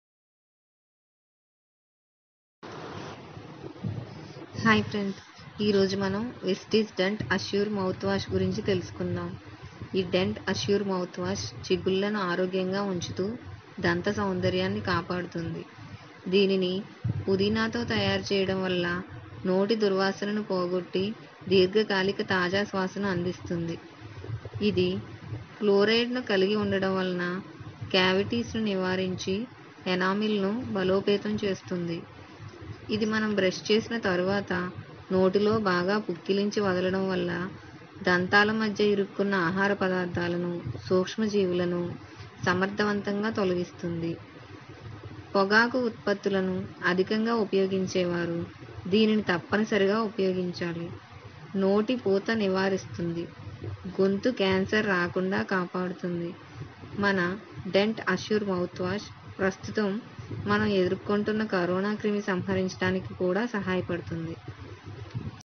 Dent Assure Mouth wash (Telugu)
Dentassure Mouthwash helps to maintain healthy gums and protects against plaque formation. It has refreshing mint flavour which provides long lasting fresh breath and helps to eliminate germs responsible for bad breath. It also maintains the aesthetics and health of teeth.
Freshens breath - Prevents bad breath for longer duration and leaves a minty fresh flavour all day long.
Prevents cavities - With regular use of mouthwash alongside brushing and flossing can reduce the chances of forming cavities. It also contains fluoride that helps to prevent cavities and strengthen the enamel.
Removes particles - Mouthwash can also be used to rinse out any loose particles in the mouth making the brushing and flossing more effective.
Although it prevents the build up of plague, it cannot reduce the plaque that already exists on the teeth, so, brushing and flossing should be done on regular basis (twice a day).
Prevents Plaque build-up - It helps prevent plaque formation on the gums, in-between teeth, and on the surface of the teeth.